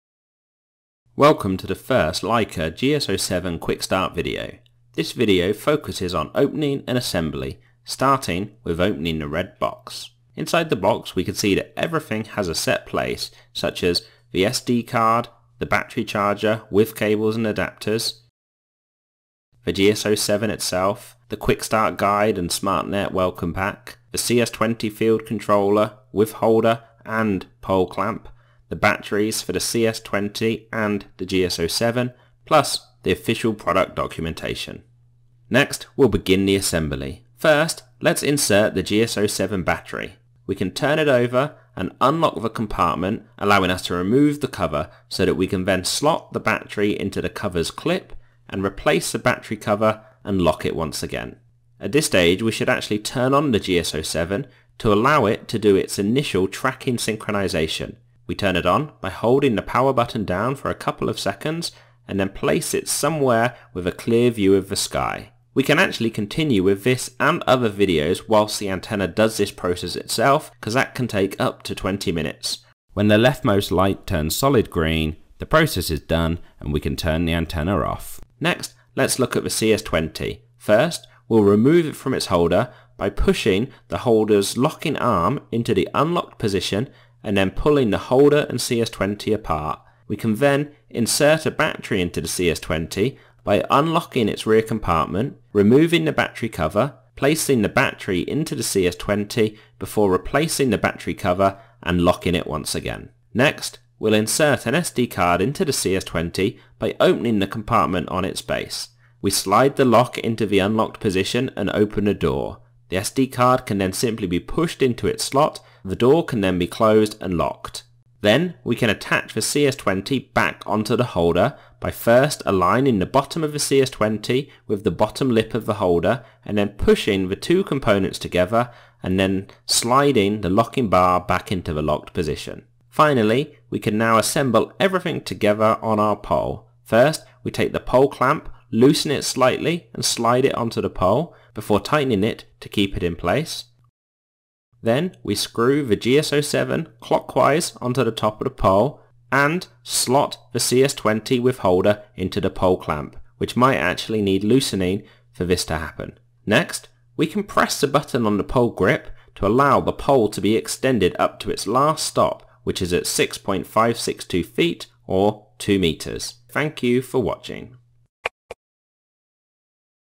Leica GS07 – 01: Opening and Assembly
The GS07 is Leica Geosystems’ new mid-range GNSS smart antenna. This video focuses on opening and assembling the instrument – box content, how to insert the battery and how to prepare for using the GS07 (including the CS20 controller).